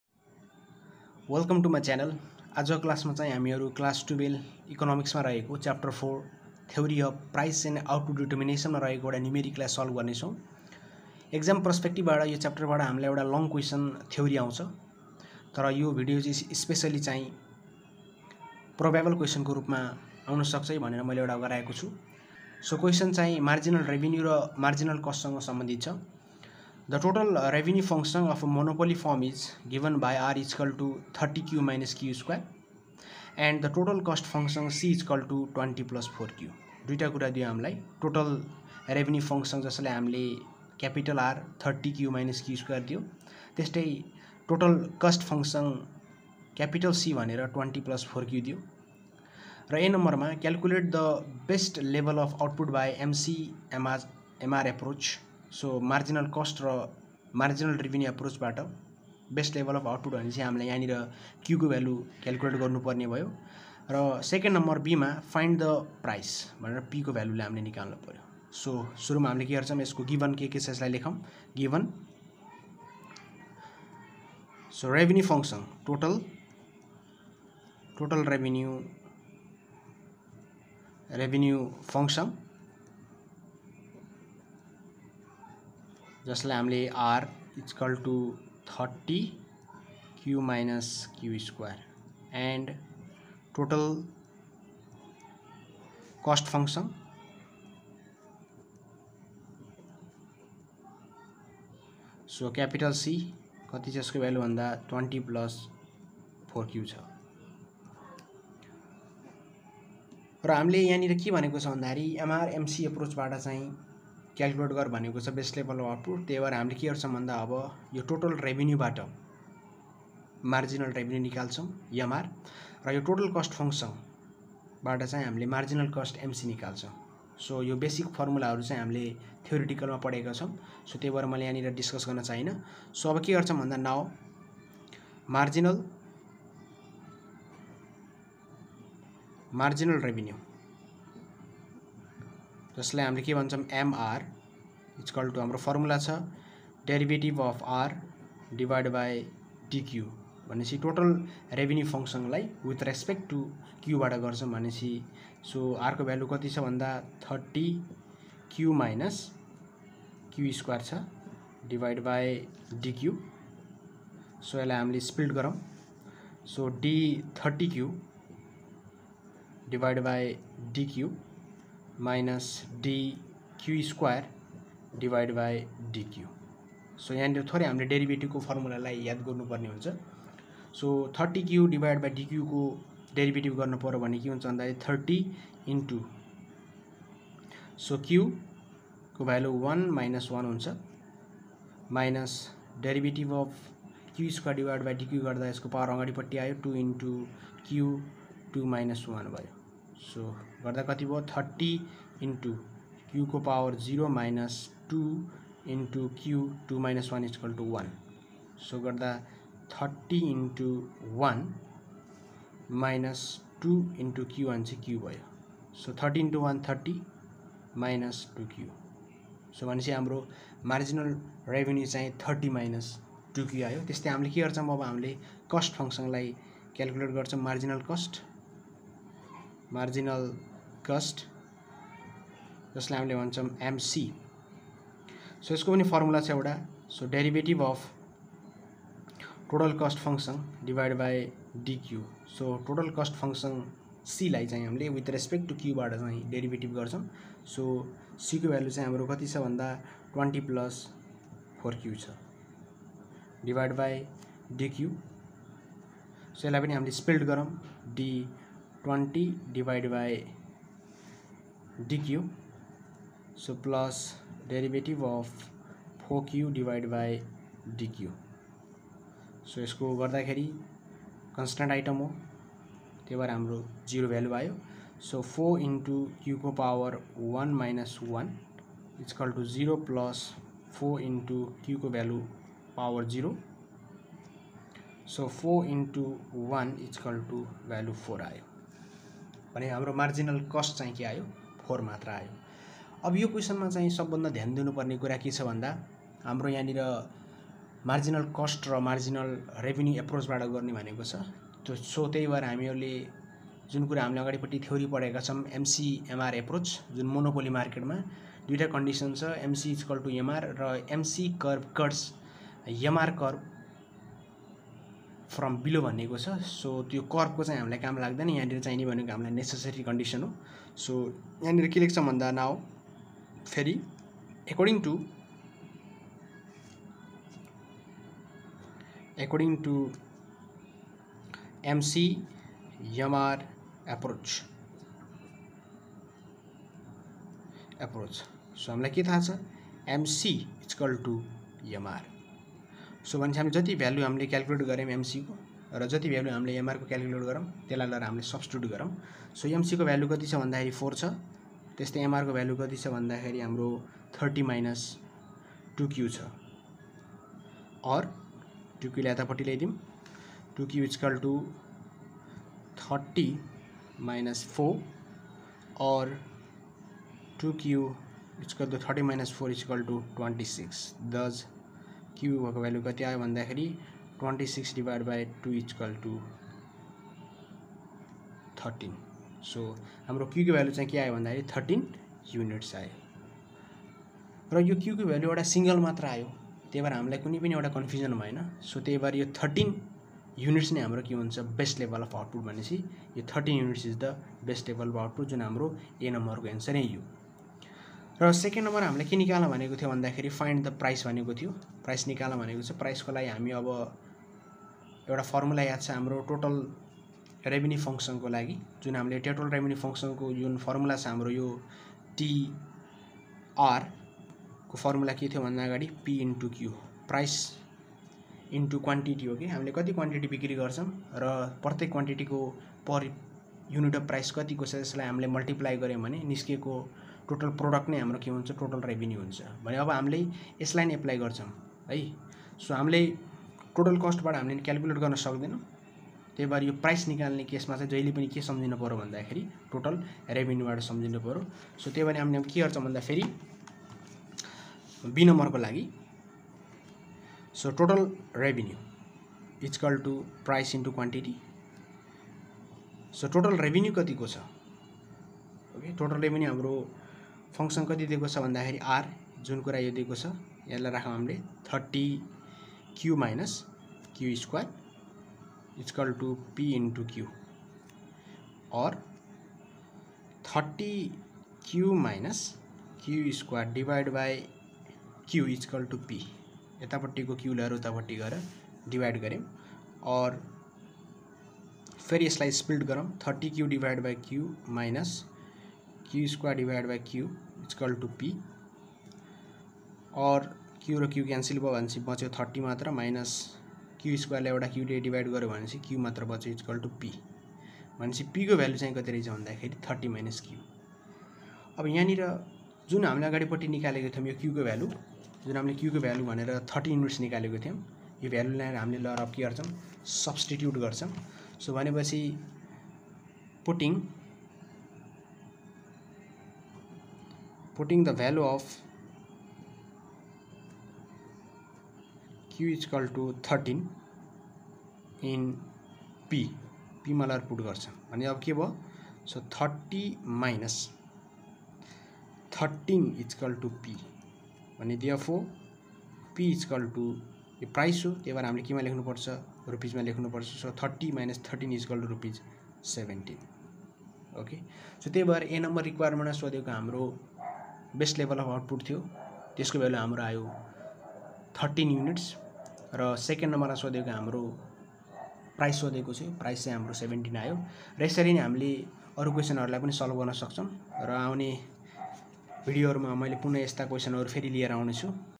Class-12| Theory of price and output determination
This video is all about class 12; Theory of price and output determination chapter numerical question.
Calculation of best level of. output and price using MC- MR approach.